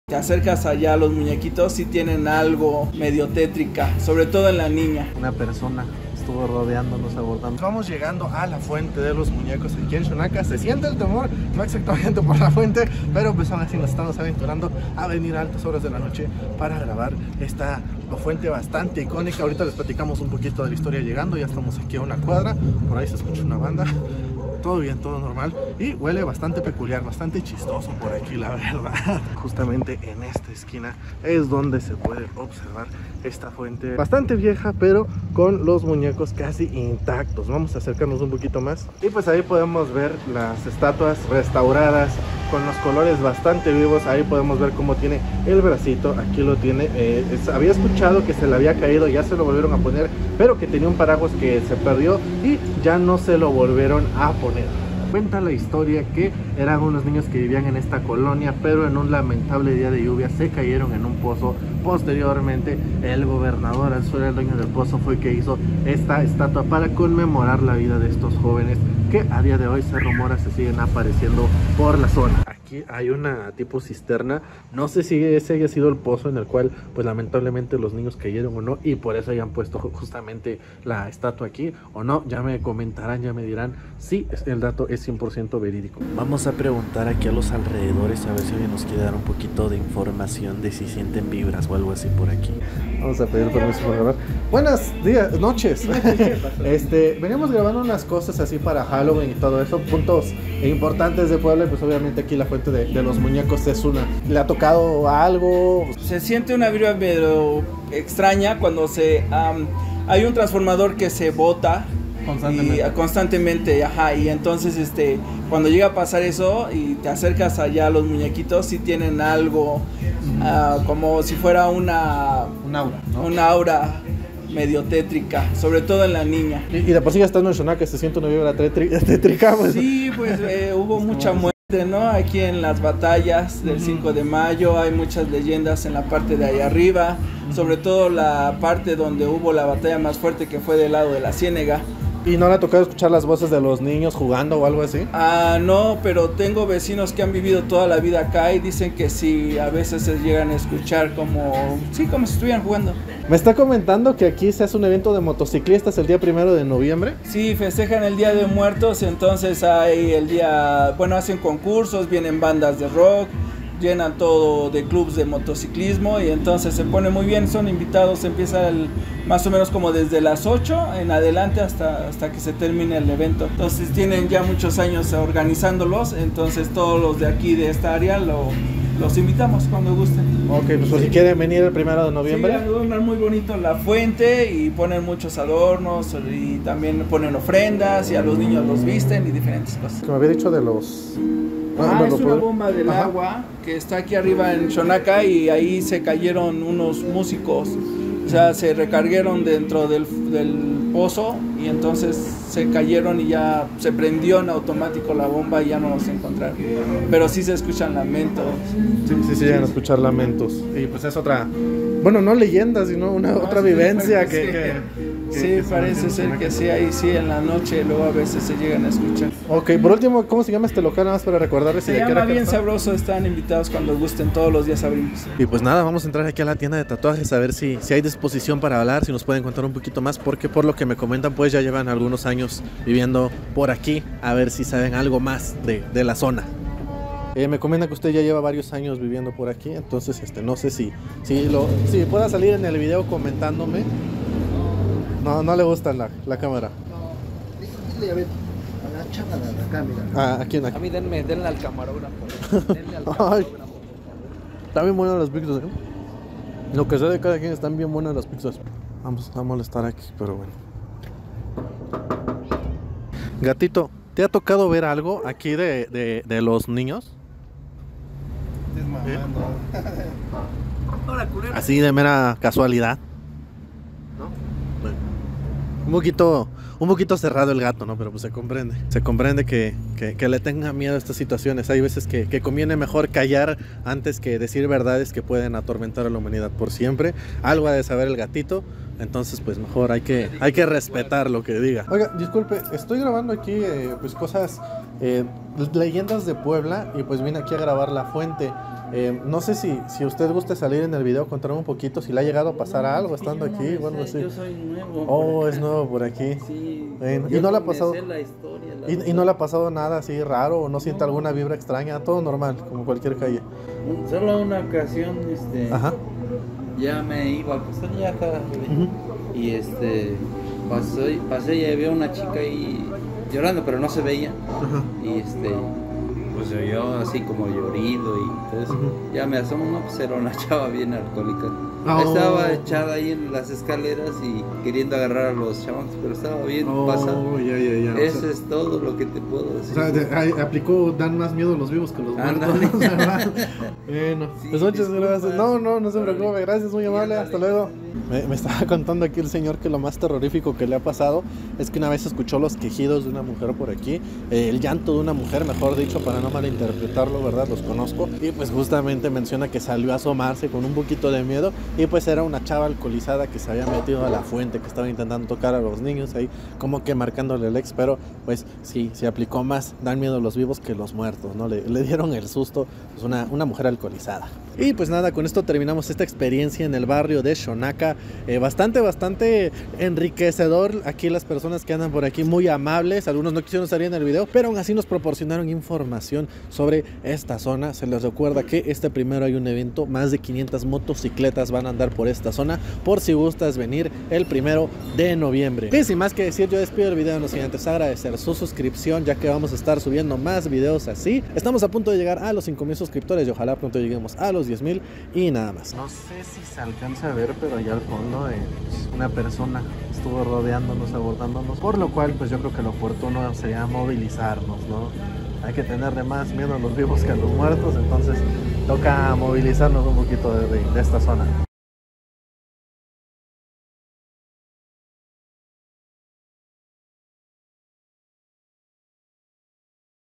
Visitamos la fuente de los Muñecos | Xonaca Puebla
Agradecimientos especiales a Pablo, Juan, David y Omar por apoyar a la grabacion de este vlog